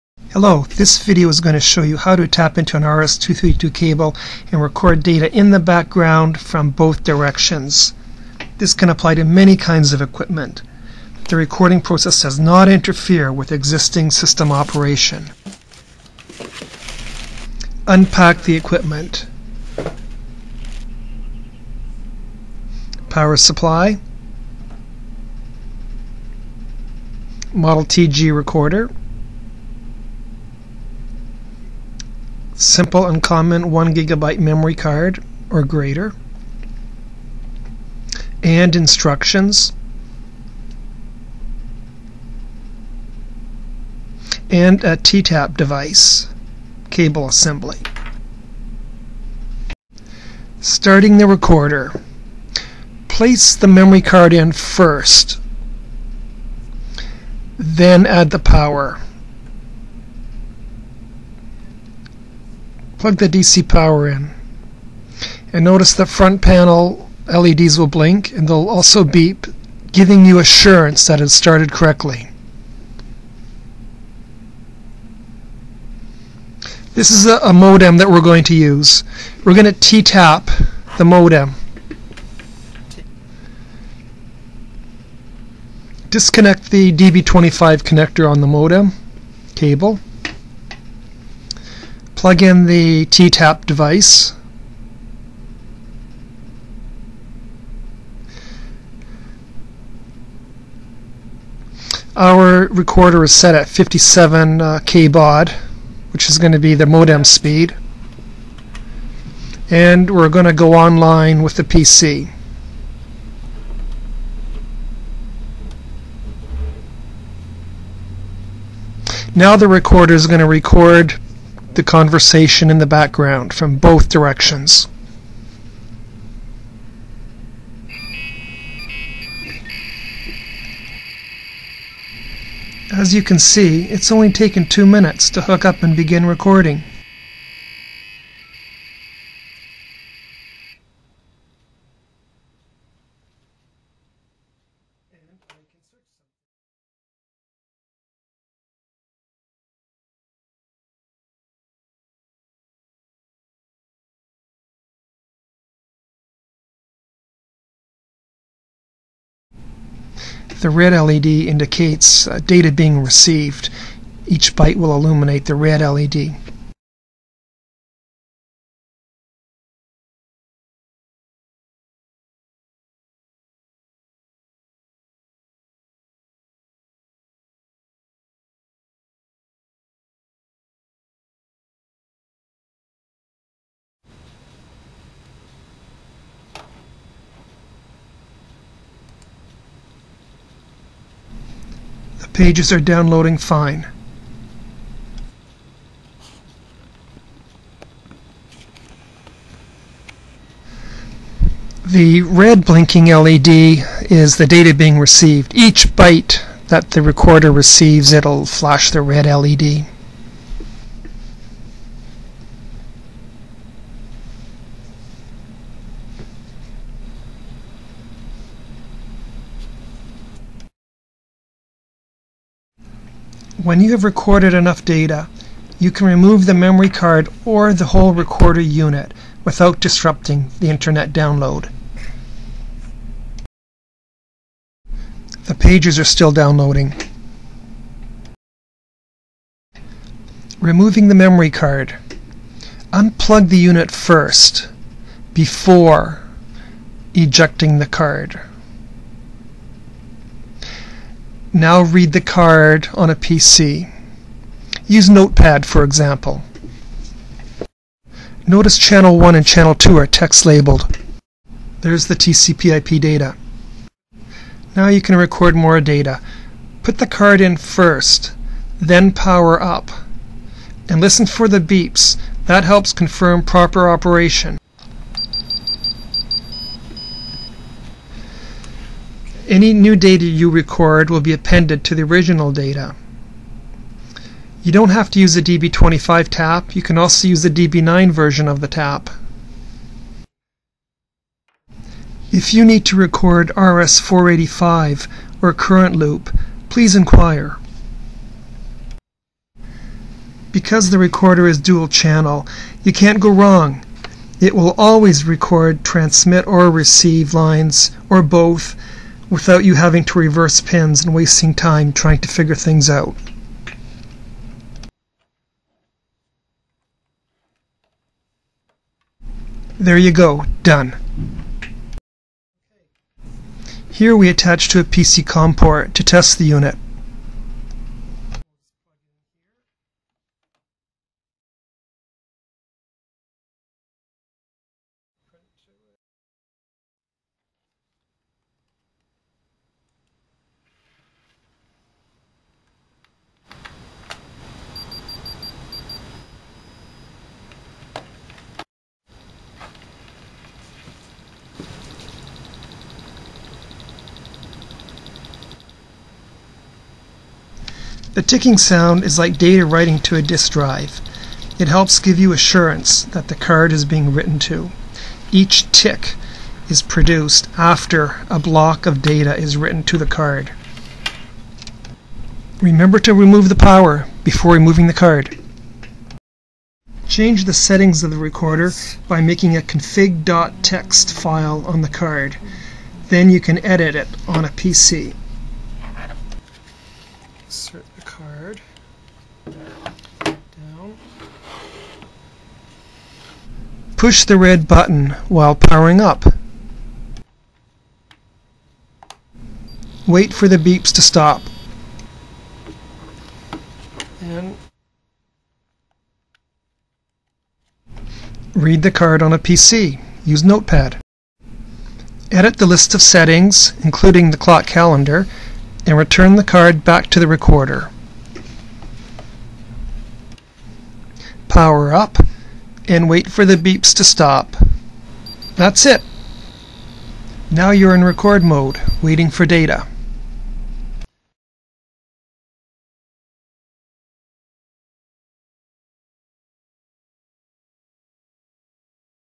Model TG Dual Channel Serial Logger With Timestamp
Model TG Serial Data Recorder demonstration.